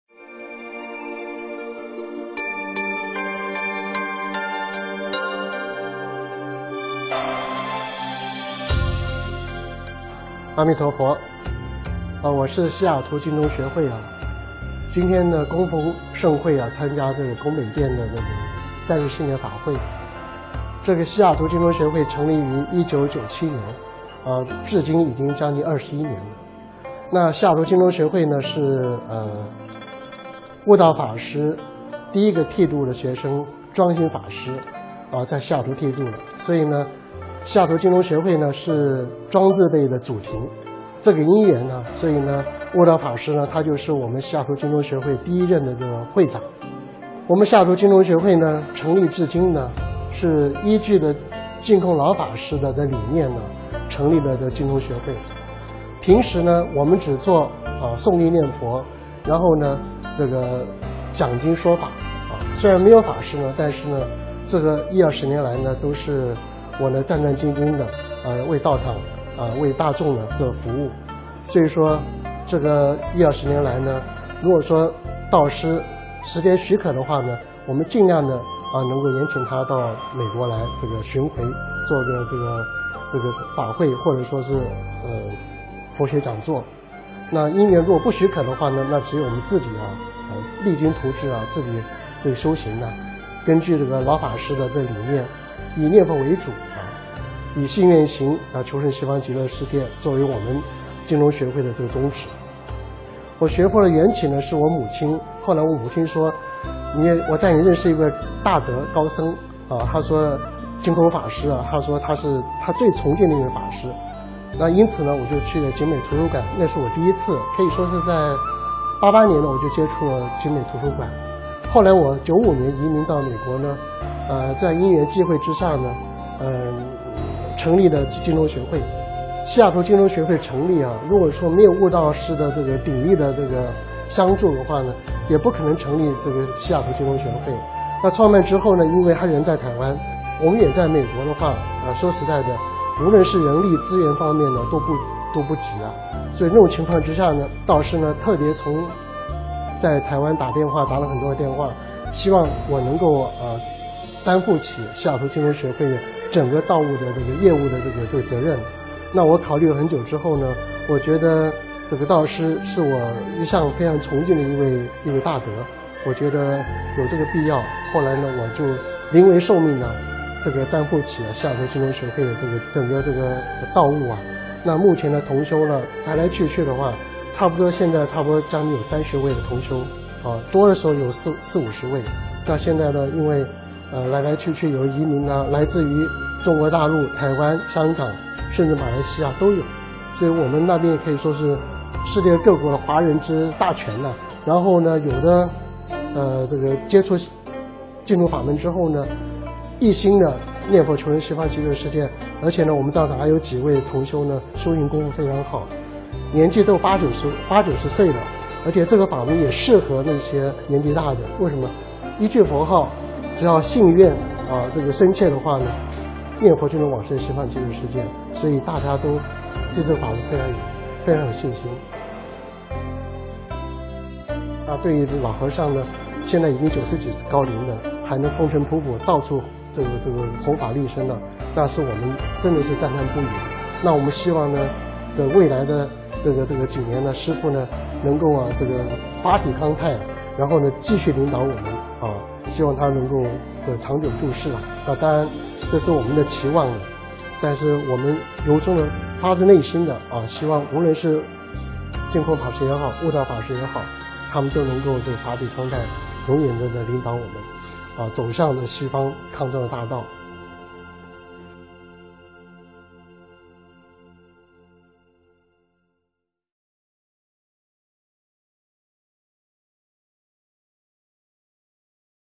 淨空老法師週年紀念:恩師上淨下空老和尚弘法60周年暨華藏淨宗學會成立30周年各淨宗單位訪談祝賀影片（第九集） {最新*鏈接有字幕快覩版}
~其他影音-其他--影片..{恩師淨公老和尚~美國西雅圖淨宗學會專訪}(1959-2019)...
~檔名：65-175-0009，容量：8.6MB-全1集*(上載實時：28/05/2019, 10:11:00)
~日期：05~06'2019，（共一集）地點講於:華盛頓州金郡,台灣華藏淨宗弘化網製作

影片字幕：(無文字稿,先覩為快版)
"恩師上淨下空老和尚弘法60周年暨華藏淨宗學會成立30周年各淨宗單位訪談祝賀影片（第九集）"
{以下*有影片字幕~細讀先覩版鏈接網}：~